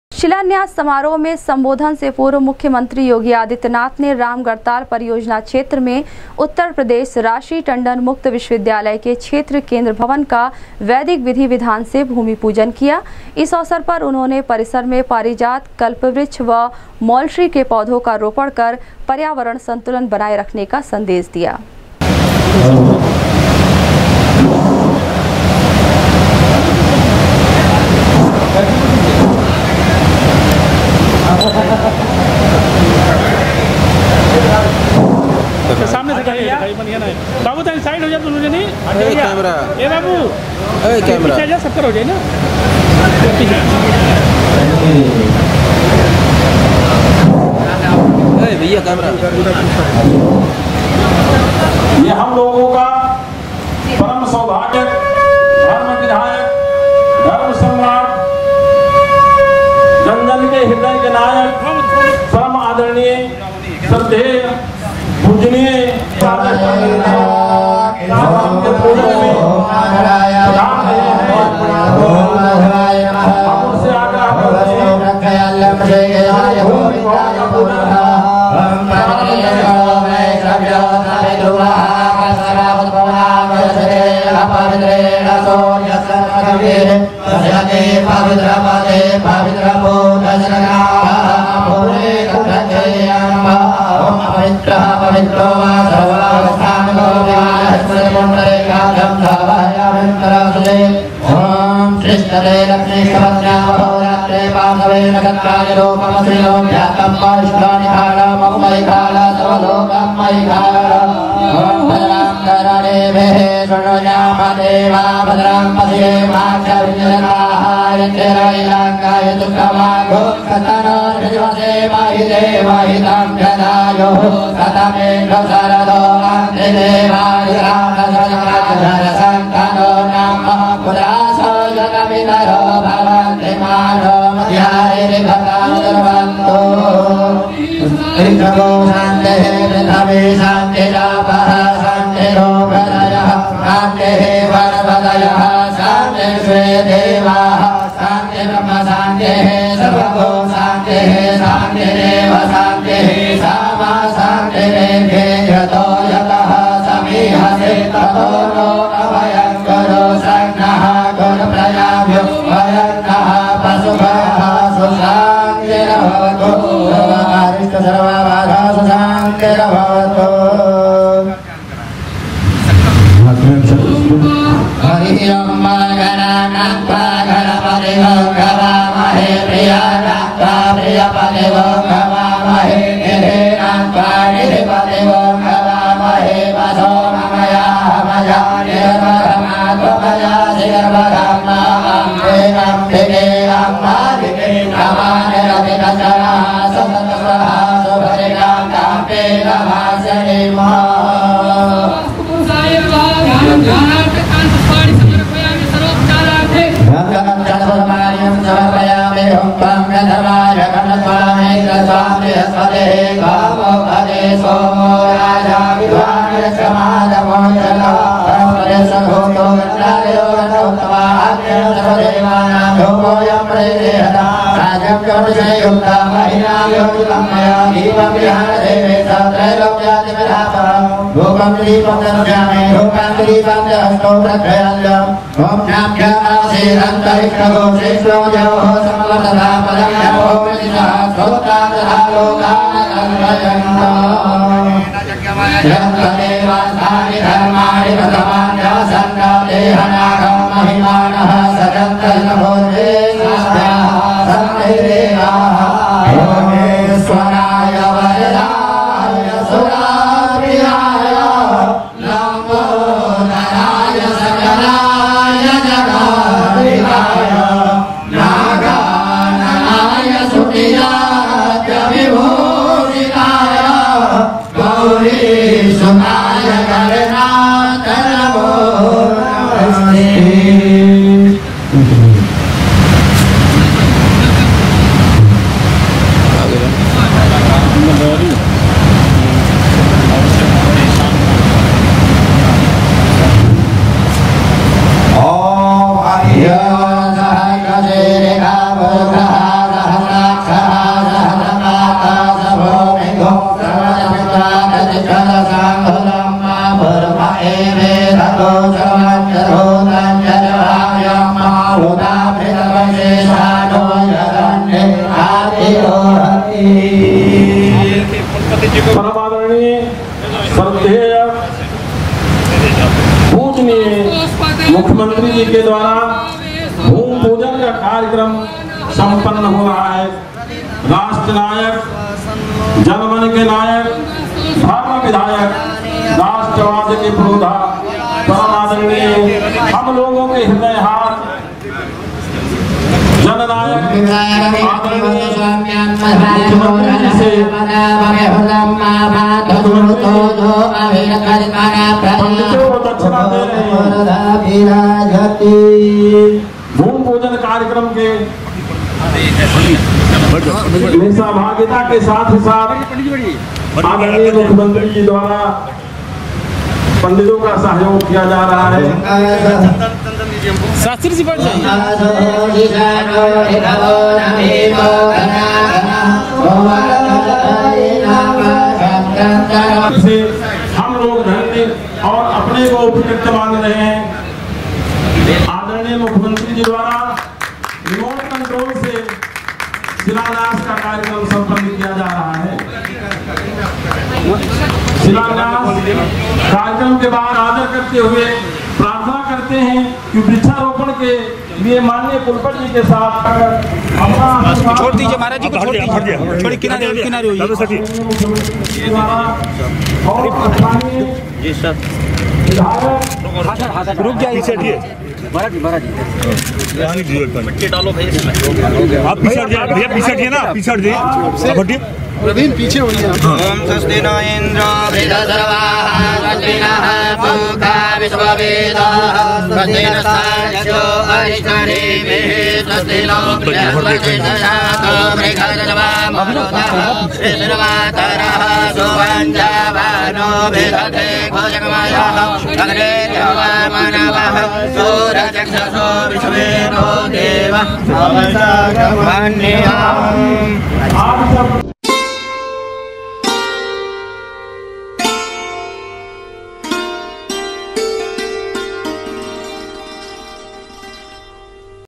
उत्तर प्रदेश राजर्षि टंडन मुक्त विश्वविद्यालय के गोरखपुर क्षेत्रीय केंद्र भवन का भूमि पूजन कर सीएम
उत्तर प्रदेश राजर्षि टंडन मुक्त विश्वविद्यालय के गोरखपुर क्षेत्रीय केंद्र भवन का भूमि पूजन कर सीएम ने पारिजात, कल्पवृक्ष व मौलश्री के रोपे पौधे, दिया पर्यावरण संतुलन का बनाये रखने का संदेश